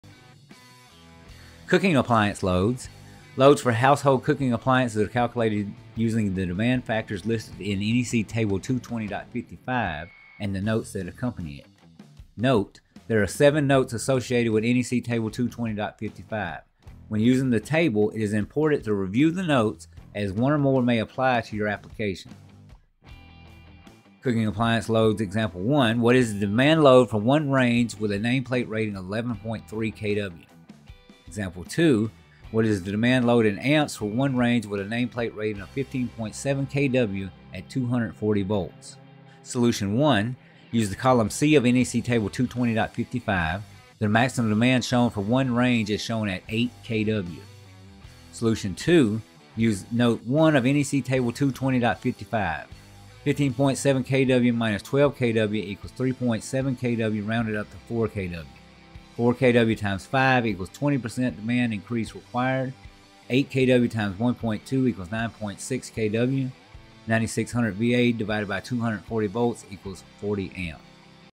How do you Calculate Cooking Equipment Loads using the NEC notes?
🔌🍳📝 Explore the art of calculating cooking equipment loads using the NEC notes in this informative video! Learn how to ensure your kitchen appliances are safely powered while adhering to NEC standards.